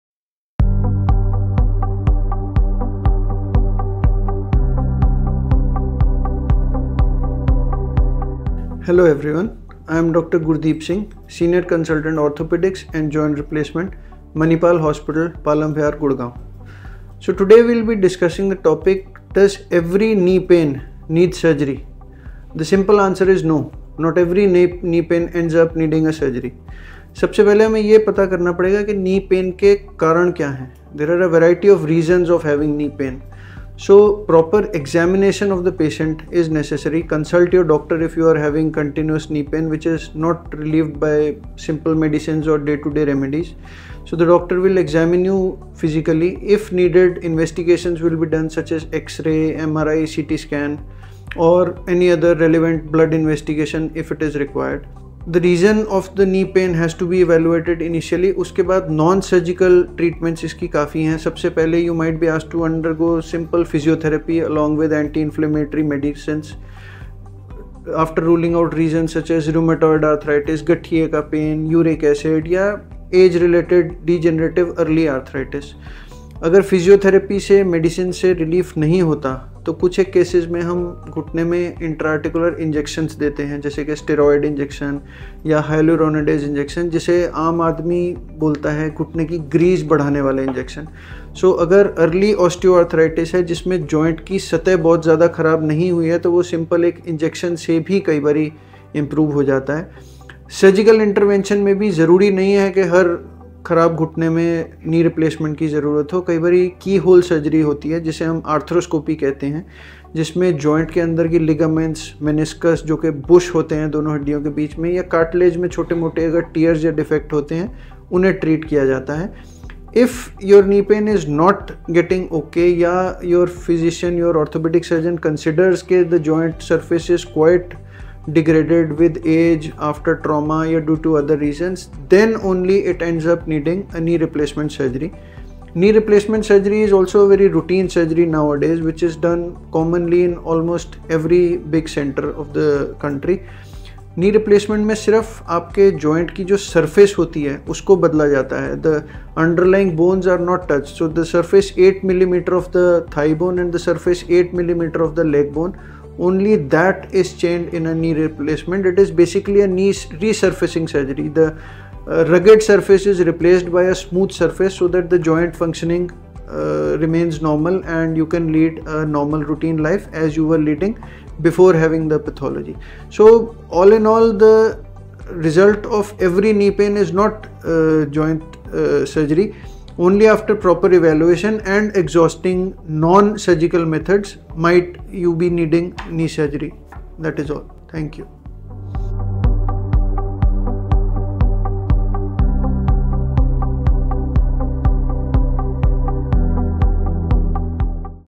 Knee Pain Surgery | Manipal Hospital Gurugram | Dr. Avinash Ratra
In this video, Dr. Gurdeep Avinash Ratra, a Senior Consultant in orthopaedics at Manipal Hospital Gurugram, discusses whether every case of knee pain necessitates surgery. He emphasizes that the answer is no; not all knee pain requires surgical intervention. It's crucial to first determine the underlying causes, as there are various types of knee pain that warrant different approaches. Dr. Ratra also highlights the various treatment options available.